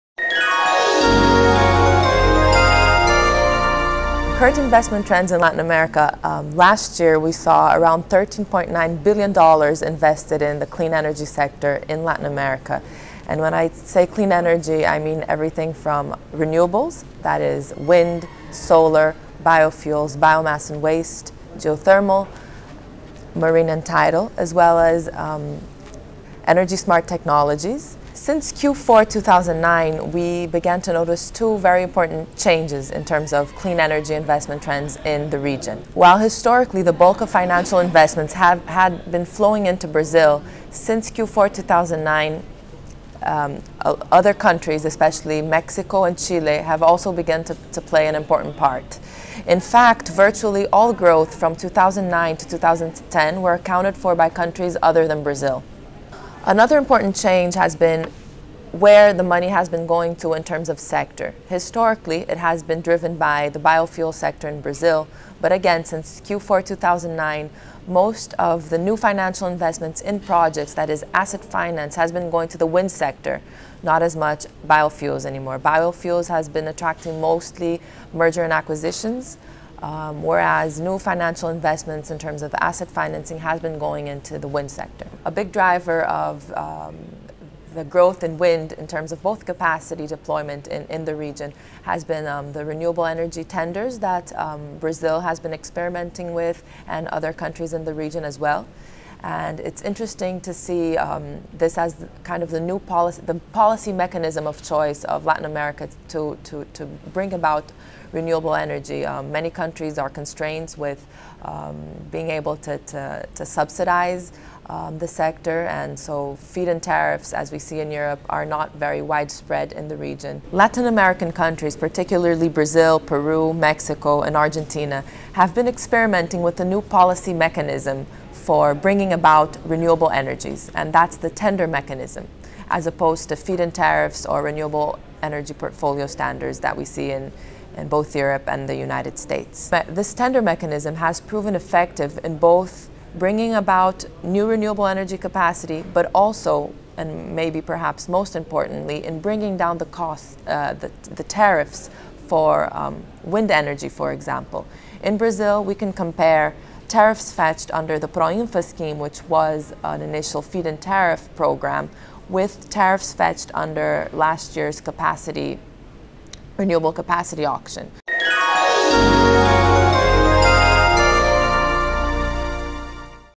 CSRAméricas 2011 › Clean Energy in Latin America
Bloomberg's María Gabriela Da Rocha explains how is the market for clean energy in Latin America and the Caribbean. Brazil is becoming a key market but the industry needs to face some challenges to grow and become sustainable. One of them? Tariffs.

MicAmericas TV at CSRAméricas 2011. Asunción, Paraguay. Recorded on May 26th, 2011.

Executive Producer: Diego Fonseca / Producer: Andrea San Juan / Video Editor: Juan Franco.